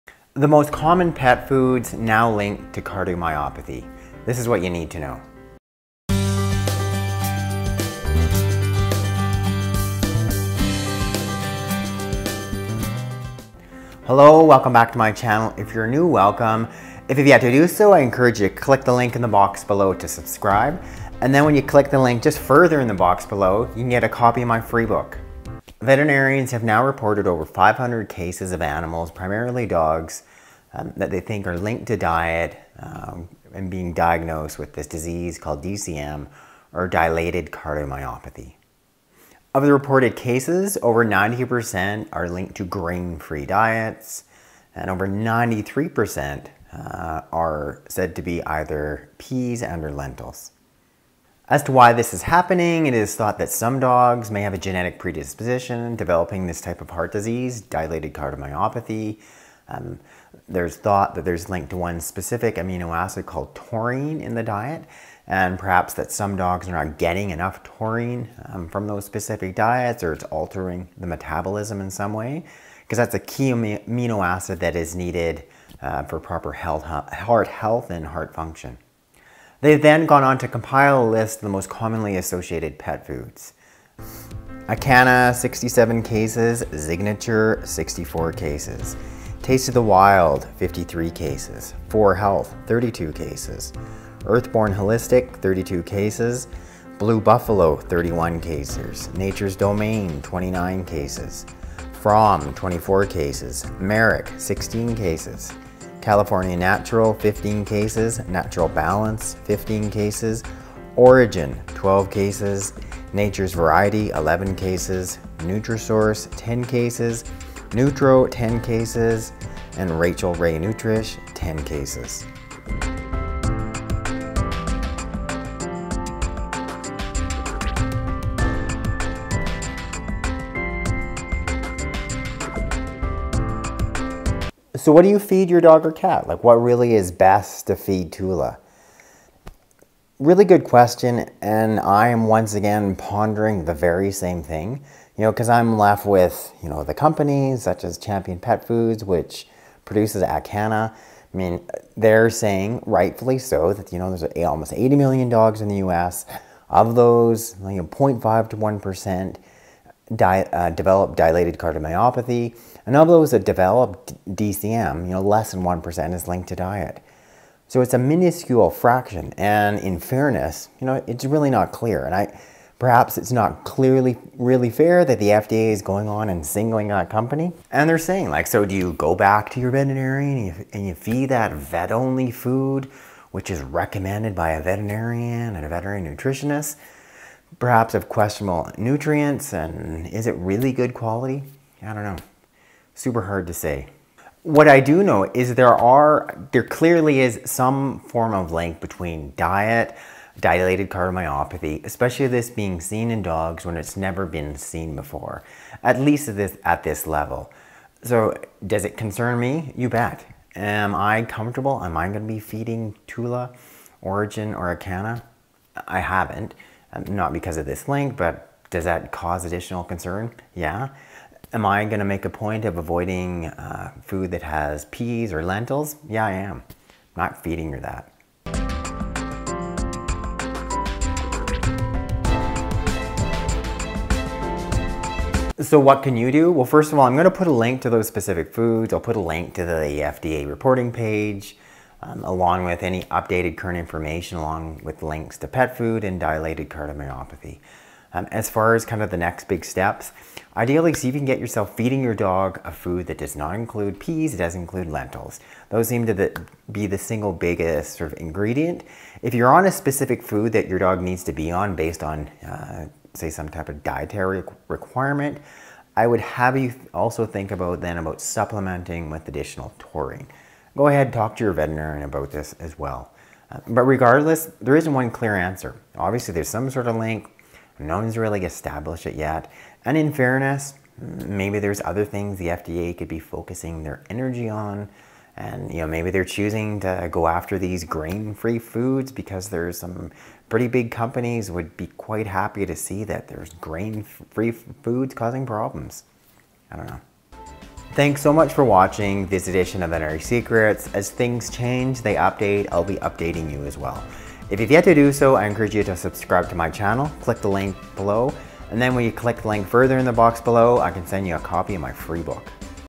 Pet Foods Linked To Heart Disease (16 Brands)
📘 Dr. Jones' Free Book: Unlock veterinary secrets with Dr. Jones' insightful book on animal health care.

The FDA has released their findings into the investigation of Diet and DCM ( Dilated Cardiomyopathy), reporting 524 cases, and 16 Brands of Pet Food which have been linked to 10 cases or more.

• Acana (67 cases)
• Zignature (64 cases)
• Taste of the Wild (53 cases)
• 4Health (32 cases)
• Earthborn Holistic (32 cases)
• Blue Buffalo (31 cases)
• Nature’s Domain (29 cases)
• Fromm (24 cases)
• Merrick (16 cases)
• California Natural (15 cases)
• Natural Balance (15 cases)
• Orijen (12 cases)
• Nature’s Variety (11 cases)
• NutriSource (10 cases)
• Nutro (10 cases)
• Rachael Ray Nutrish (10 cases)

Both pet owners and veterinary professionals should continue to report cases of dogs and cats with DCM that could be linked to diet. To report, use the online Safety Reporting Portal

In this crucial update, Dr. Andrew Jones delves into the recent FDA findings on the link between certain pet foods and Dilated Cardiomyopathy (DCM), a serious heart disease affecting dogs. The report outlines 524 cases and highlights 16 brands associated with 10 or more incidents. Discover which brands are on the list, understand the potential risks, and learn what steps you can take to protect your pet's heart health.